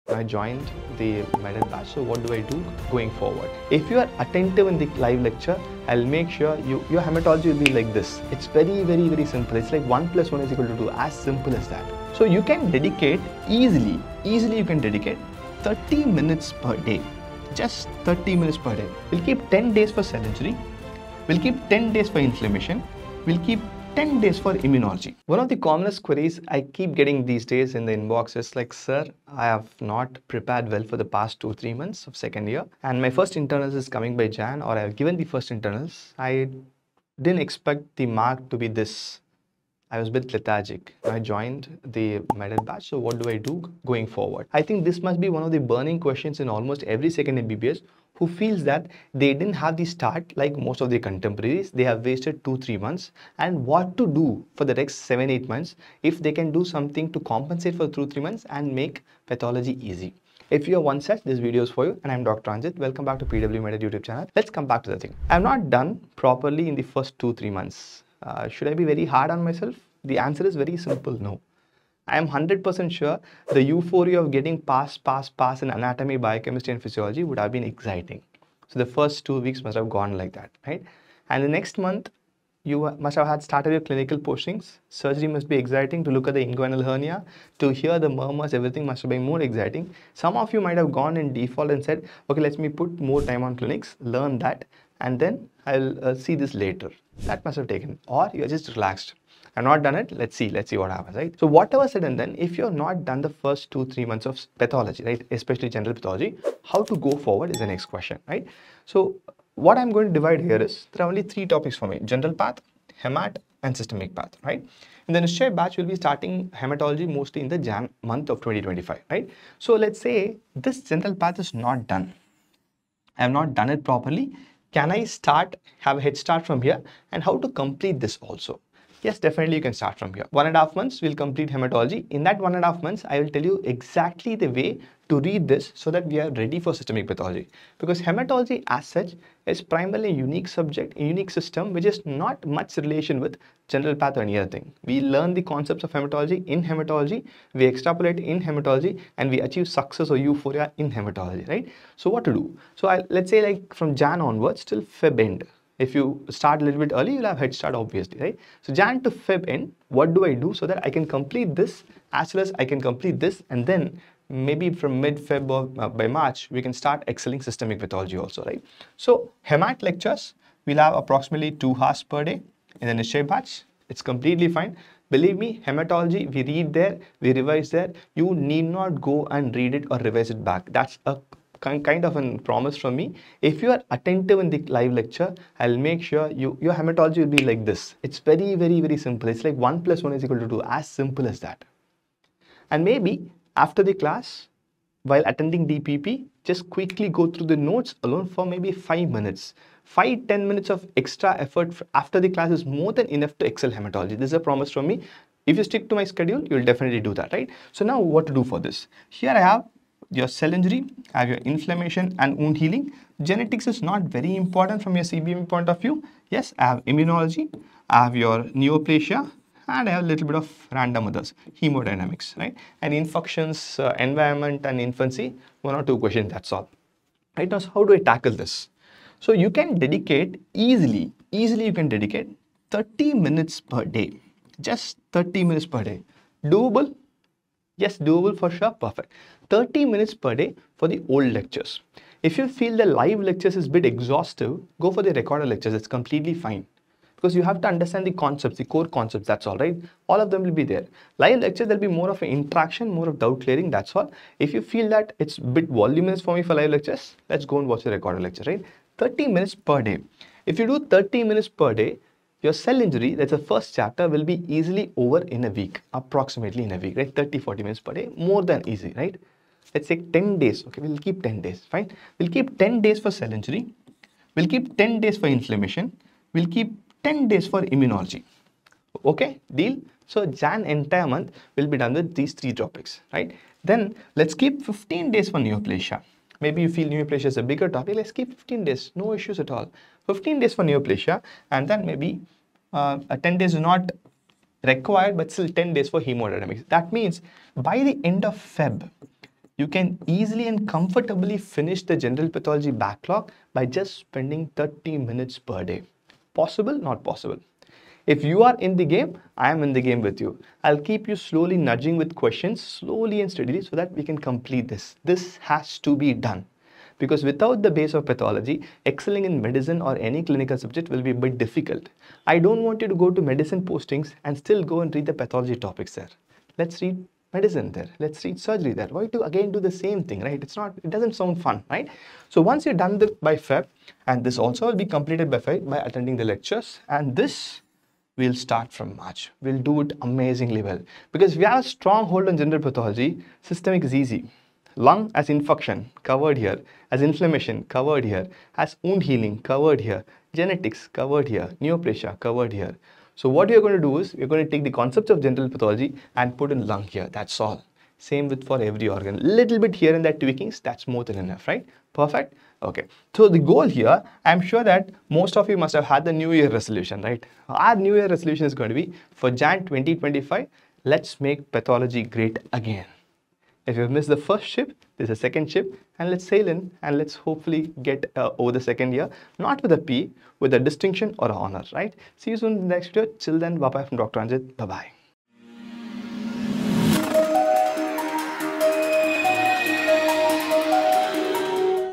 How To Read Pathology in Second Year? 2nd Year MBBS | Dr. Ranjith
Struggling to figure out the best way to study Pathology in your 2nd year of MBBS? 🩺 Dr. Ranjith is here to share practical tips and proven strategies to help you master this crucial subject.

In this video, you'll learn:
👉 How to approach Pathology topics for better understanding.
👉 The importance of linking pathology with clinical cases.
👉 How to utilize standard textbooks like Robbins efficiently.
👉 Best practices for note-making and revision.
📌 PW MedEd  SOCIAL MEDIA -
📌 PHYSICS WALLAH SOCIAL MEDIA -
 OR give a Missed Call on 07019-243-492